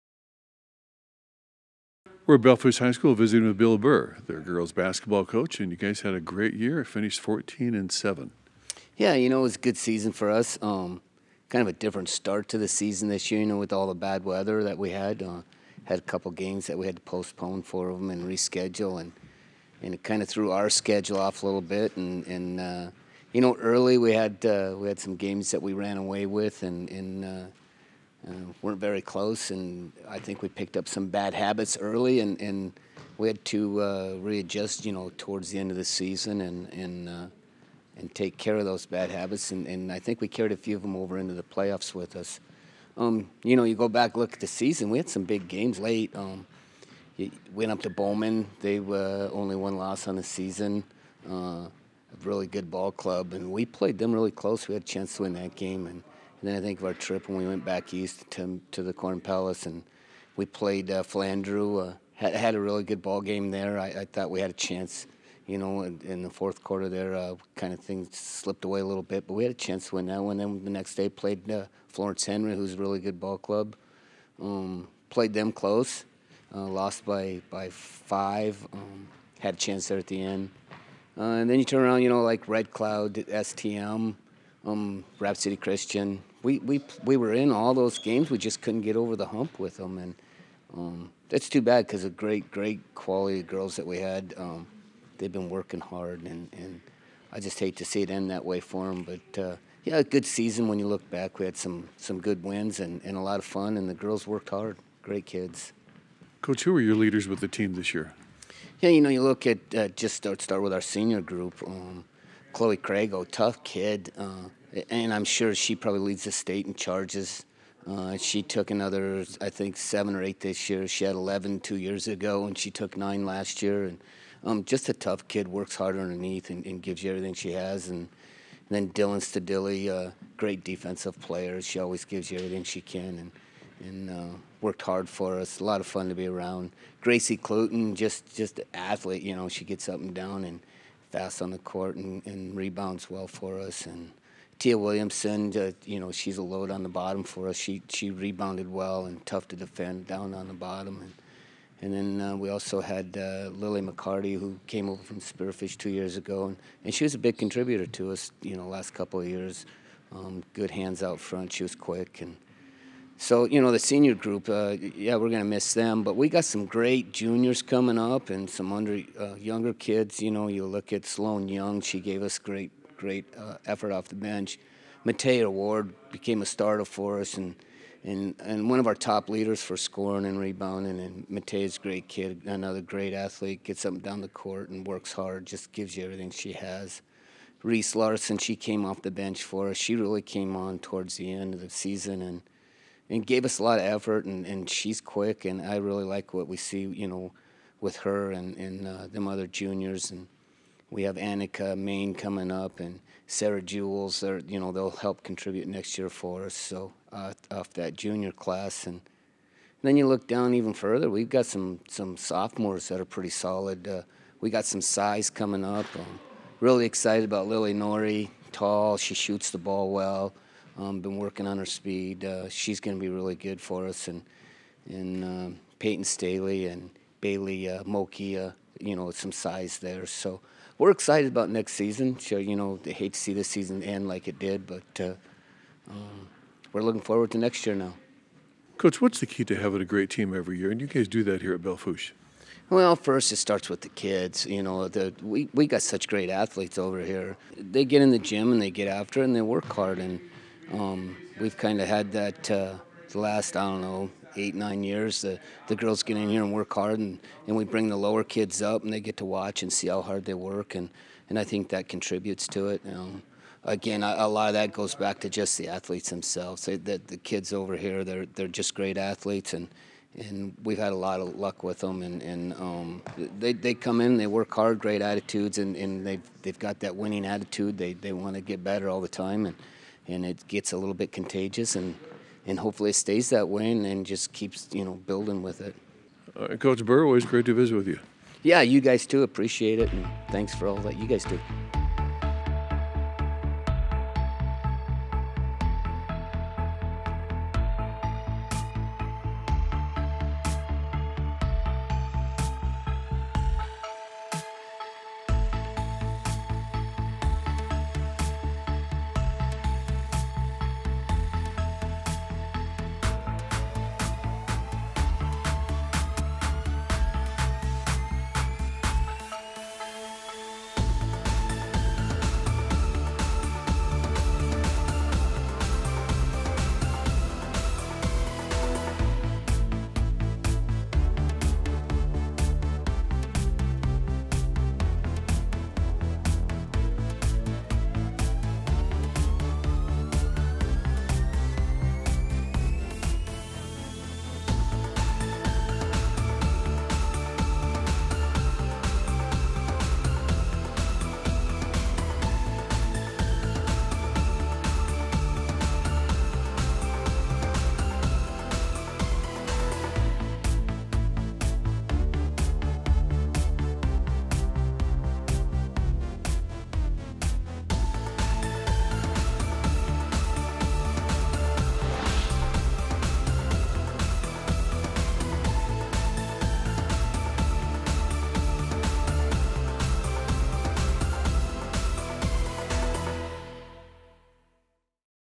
Lady Broncs post great basketball season
Belle Fourche High School girls basketball coach Bill Burr recaps the season.  Photos courtesy of Hockenbary Photos.